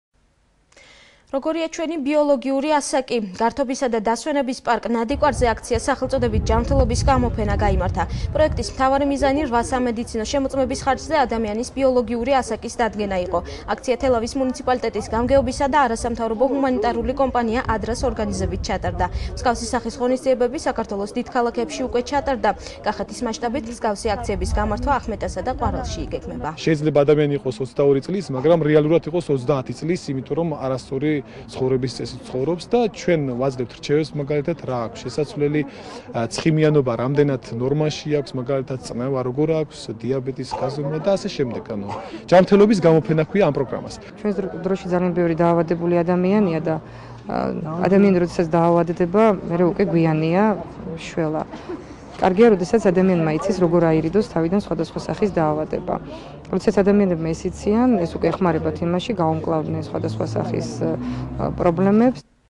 „ჯანმრთელობის გამოფენა" თელავში
როგორია  ჩვენი  ბიოლოგიური  ასაკი?  გართობისა და დასვენების პარკ „ნადიკვარზე" აქცია სახელწოდებით - „ჯანმრთელობის  გამოფენა"  გაიმართა.  პროექტის მთავარი მიზანი რვა  სამედიცინო  შემოწმების ხარჯზე ადამიანის ბიოლოგიური ასაკის  დადგენა  იყო. აქცია თელავის მუნიციპალიტეტის  გამგეობისა  და არასამთავრობო ჰუმანიტარული  კომპანია „ადრას" ორგანიზებით  ჩატარდა.  მსგავსი  სახის  ღონისძიებები  საქართველოს  დიდ  ქალაქებში  უკვე  ჩატარდა.  კახეთის  მასშტაბით,  მსგავსი  აქციების გამართვა ახმეტასა და ყვარელშიც იგეგმება.